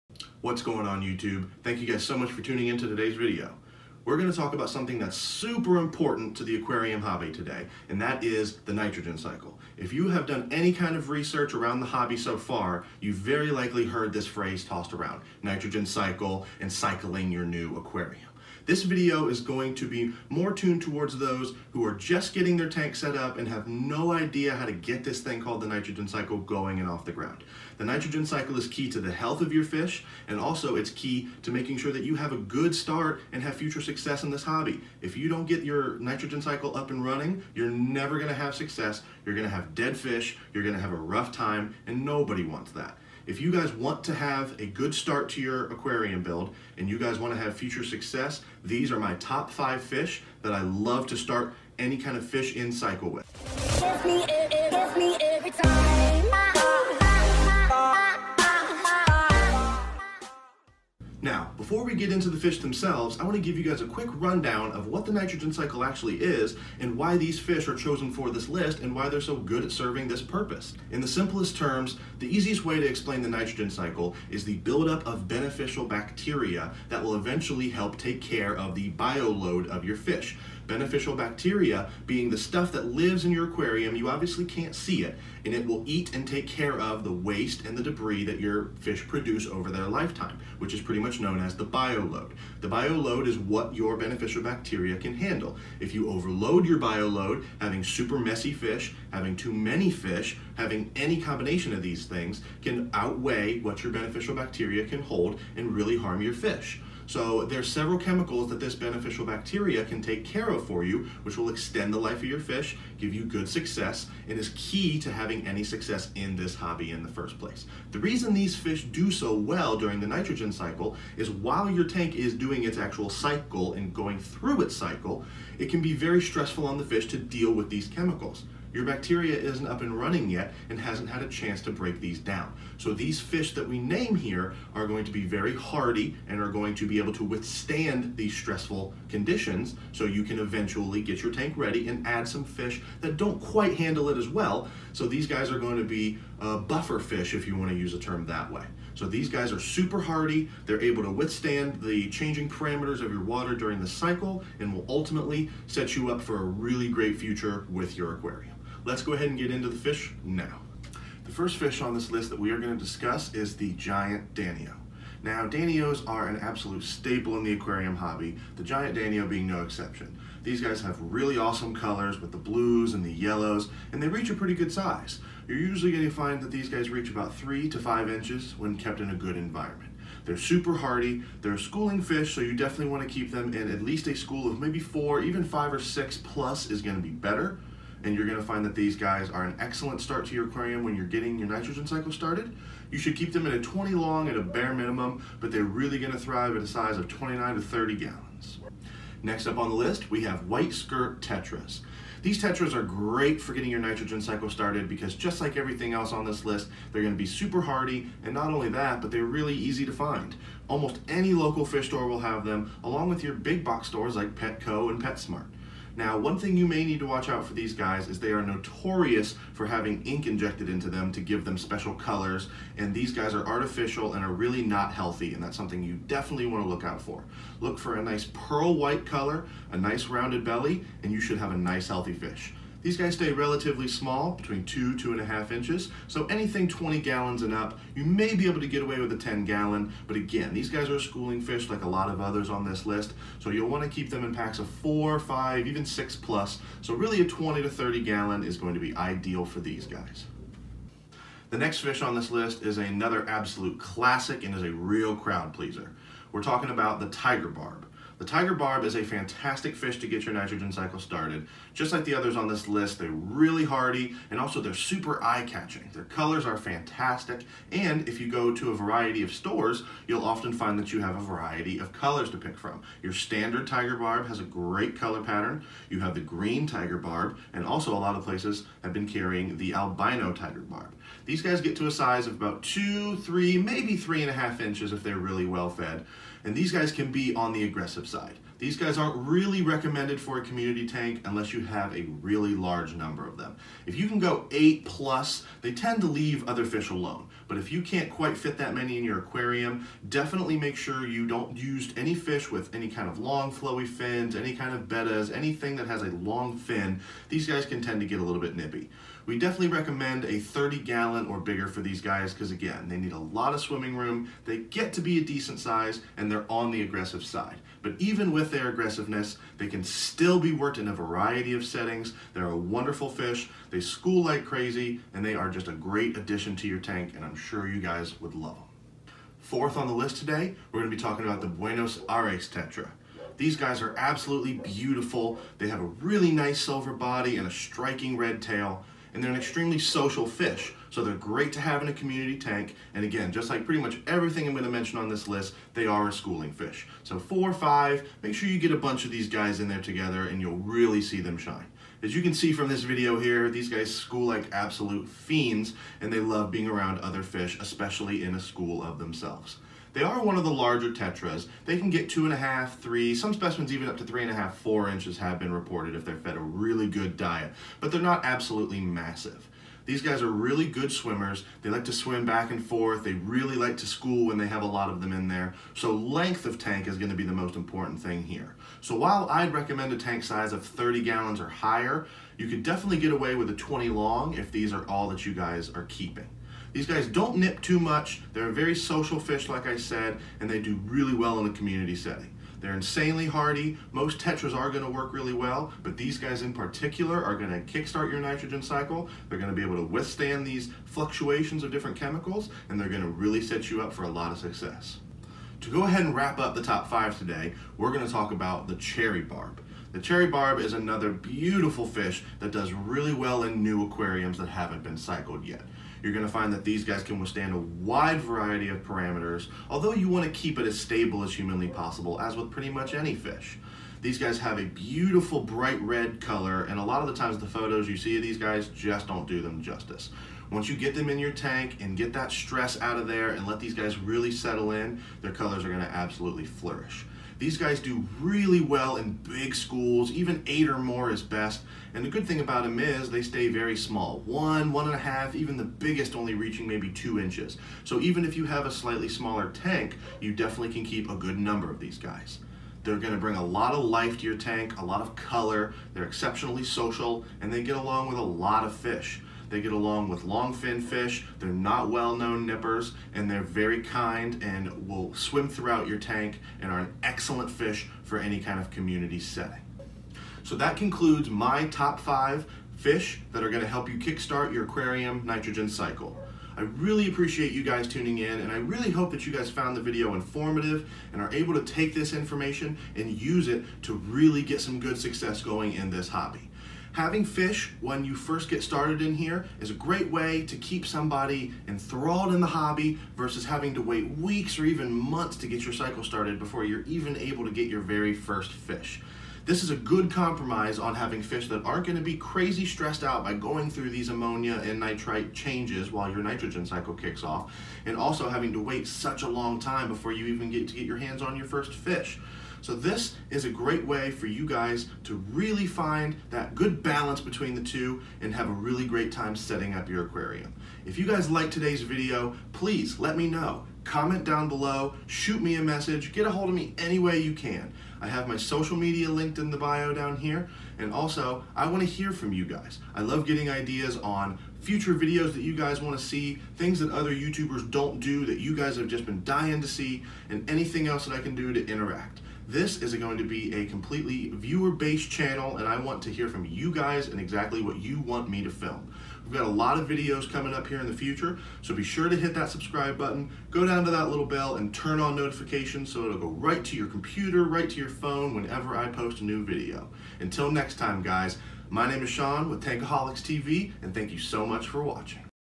CYCLING A NEW TANK? TOP 5 NITROGEN CYCLE FISH!
Wondering which fish can survive the nitrogen cycle in your aquarium? In this video I talk about 5 easy to find fish that will jump start your tanks cycle!